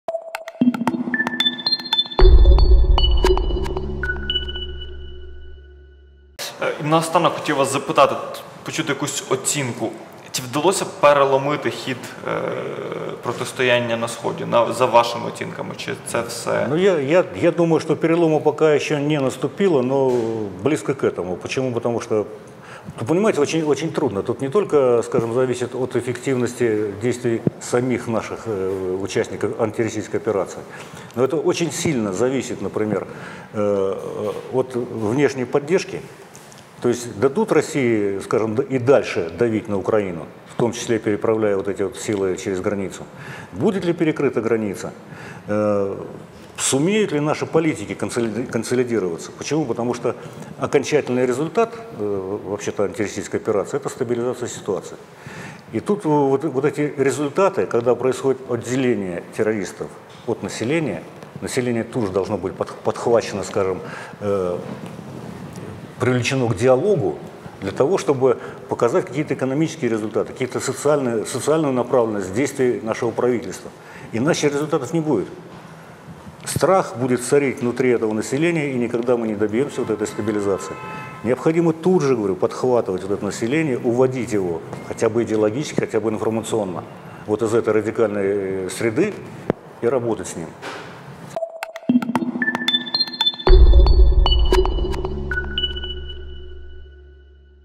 Микола Сунгуровський: Переломний момент на Сході ще не наступив
Директор військових програм Центру Разумкова Микола Сунгуровський на Громадському 06.06.14